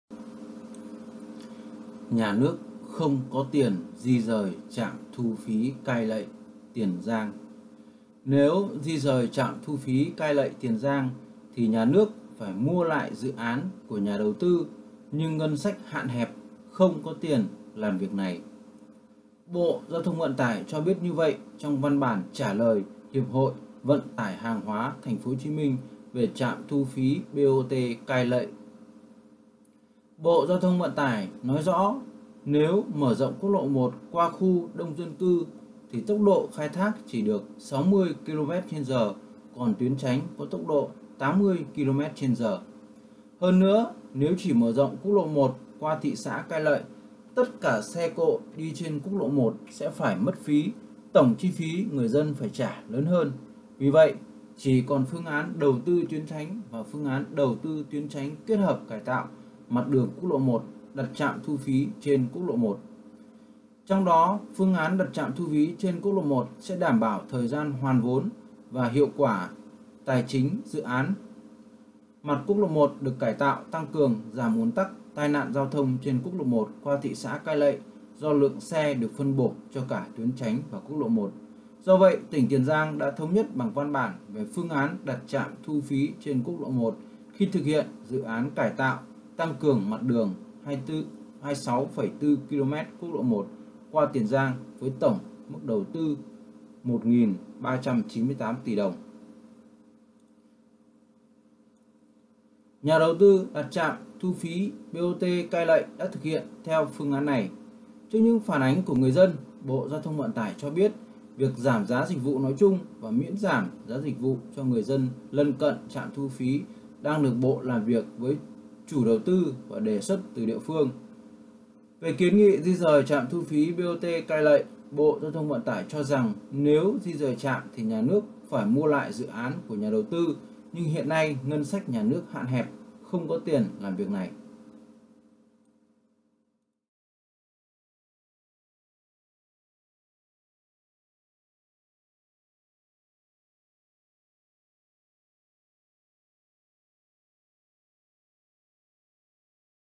Nhà nước không có tiền di dời trạm thu phí Cai Lậy, Tiền Giang. Các thượng đế phải...gánh thôi?
Nếu di dời trạm thu phí Cai Lậy (Tiền Giang) thì  Nhà nước phải mua lại dự án của nhà đầu tư, nhưng ngân sách hạn hẹp, không có tiền làm việc này.

Bộ GTVT cho biết như vậy trong văn bản trả lời Hiệp hội Vận tải hàng hóa TP.HCM về trạm thu phí BOT Cai Lậy.

Bộ GTVT nói rõ, nếu mở rộng QL1 qua khu đông dân cư thì tốc độ khai thác chỉ được 60km/h, còn tuyến tránh có tốc độ 80km/h.

Hơn nữa, nếu chỉ mở rộng QL1 qua thị xã Cai Lậy, tất cả xe cộ đi trên QL1 sẽ phải mất phí, tổng chi phí người dân phải trả lớn hơn.

Vì vậy, chỉ còn phương án đầu tư tuyến tránh và phương án đầu tư tuyến tránh kết hợp cải tạo mặt đường QL1, đặt trạm thu phí trên QL 1.

Trong đó, phương án đặt trạm thu phí trên QL1 sẽ đảm bảo thời gian hoàn vốn và hiệu quả tài chính dự án; mặt QL1 được cải tạo, tăng cường; giảm ùn tắc, tai nạn giao thông trên QL 1 qua thị xã Cai Lậy do lượng xe được phân bổ cho cả tuyến tránh và QL1.

Vì vậy, tỉnh Tiền Giang đã thống nhất bằng văn bản về phương án đặt trạm thu phí trên QL 1 khi thực hiện dự án cải tạo, tăng cường mặt đường 26,4km QL 1 qua Tiền Giang với tổng mức đầu tư 1.398 tỉ đồng.

Nhà đầu tư đặt trạm thu phí BOT Cai Lậy đã thực hiện theo phương án này.

Trước những phản ứng của người dân, Bộ GTVT cho biết việc giảm giá dịch vụ nói chung và miễn giảm giá dịch vụ cho người dân lân cận trạm thu phí đang được Bộ làm việc với chủ đầu tư và đề xuất từ địa phương.

Về kiến nghị di dời trạm thu phí BOT Cai Lậy, Bộ GTVT cho rằng nếu di dời trạm thì Nhà nước phải mua lại dự án của nhà đầu tư, nhưng hiện nay ngân sách nhà nước hạn hẹp, không có tiền làm việc này.